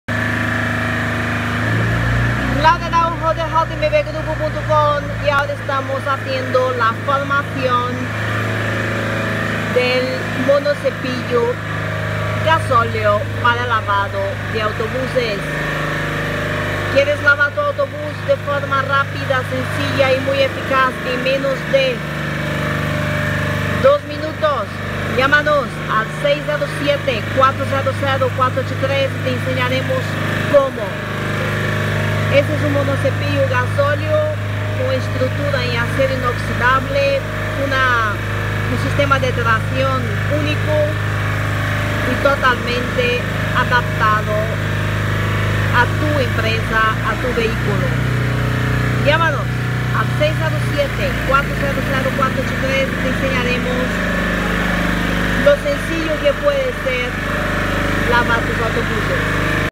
Máquina monocepillo para lavar autobuses de JMB en Logroño
Máquina monocepillo para lavar autobuses de JMB en Logroño:  Modelo diesel.  Este rodillo es totalmente autónomo, lleva depósito de agua incorporado, una tracción activa para que te resulte extremadamente ligero.

El monocepillo diesel junto con el sistema de lavado de JMB es la combinación perfecta para que puedas lavar tu autobús de forma rápida, sin ningún esfuerzo y en menos de 2 minutos.  Te lo imaginas?